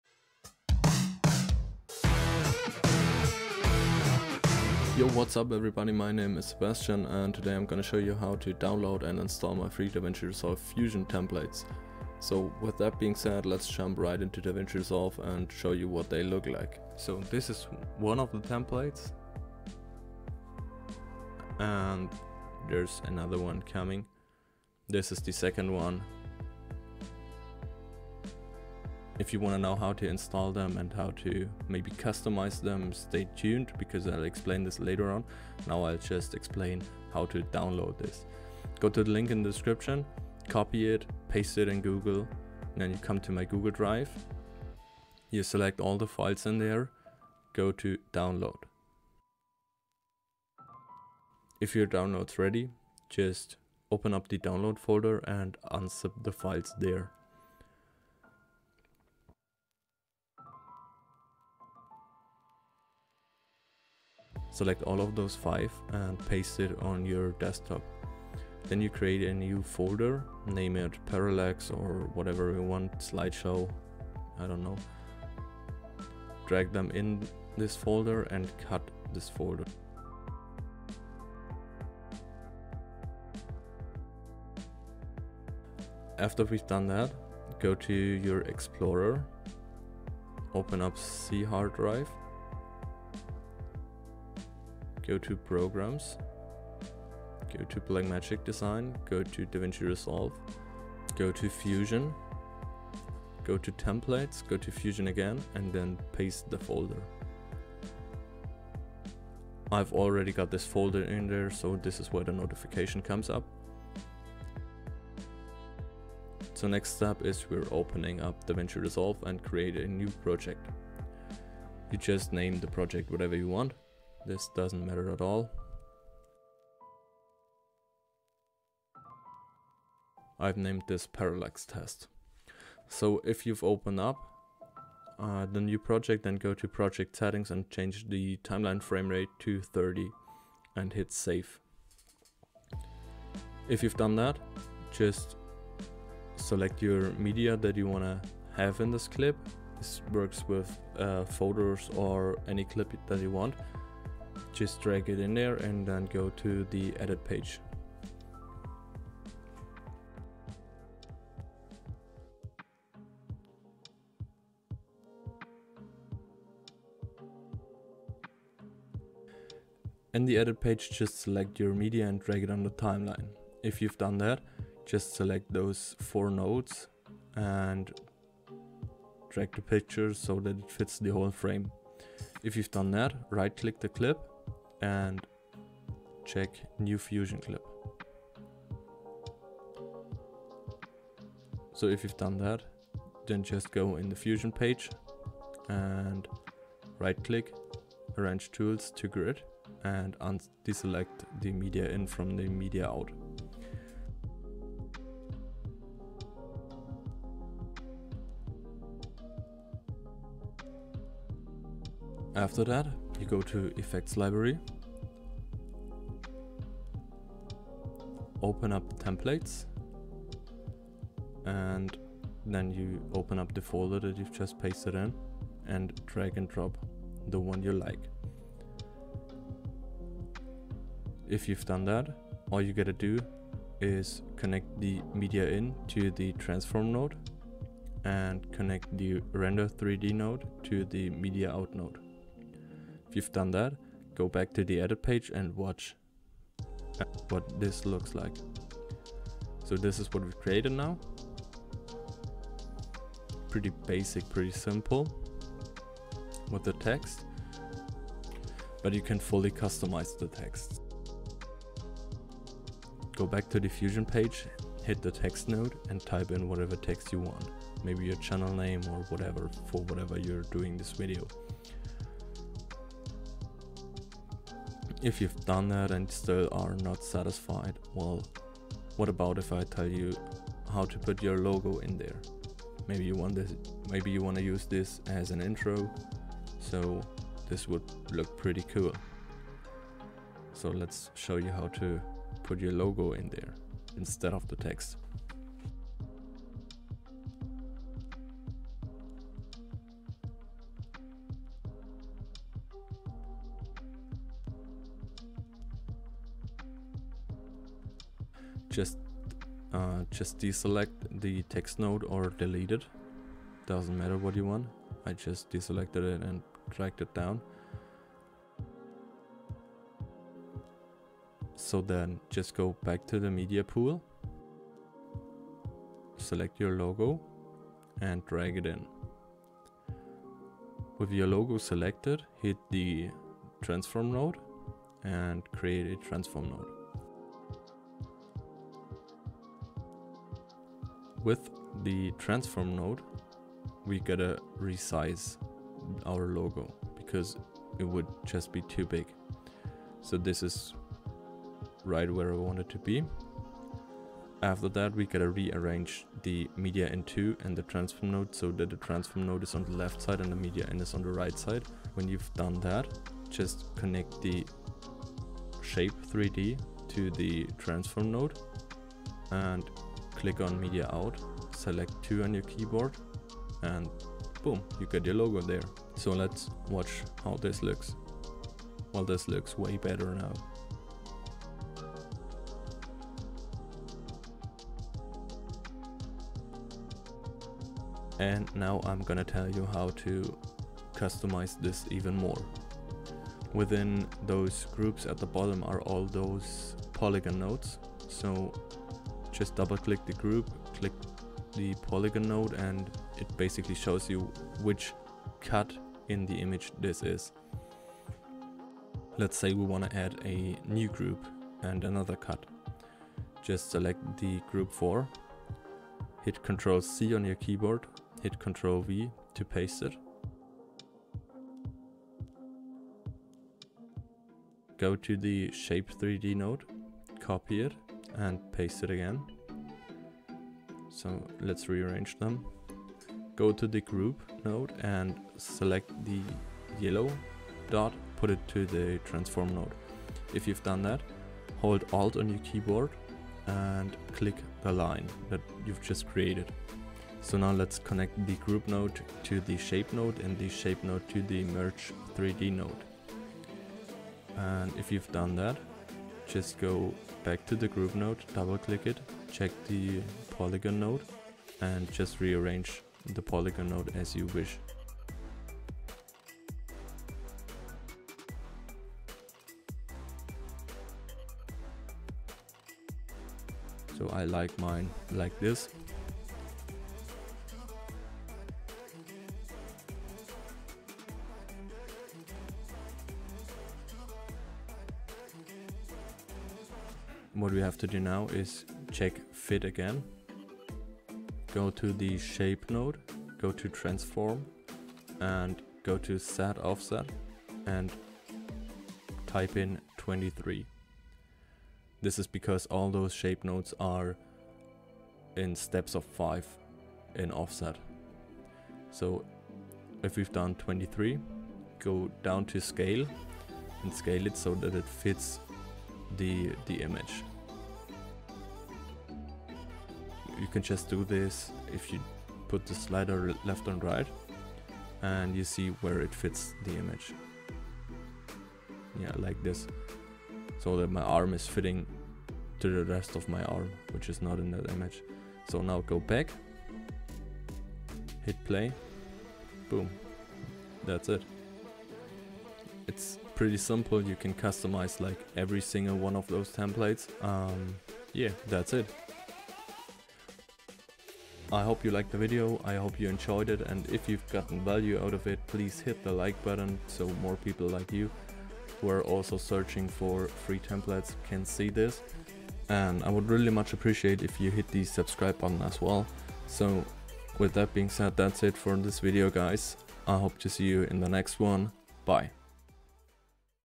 **OUTDATED** FREE Fusion Templates for DaVinci Resolve
Here you`ll get 5 FREE FUSION TEMPLATES to install right in DaVinci Resolve.
I`ll also show you how to install them so it works best for you!

Thanks guys, have a great day!

Gear I Use:
SD Card: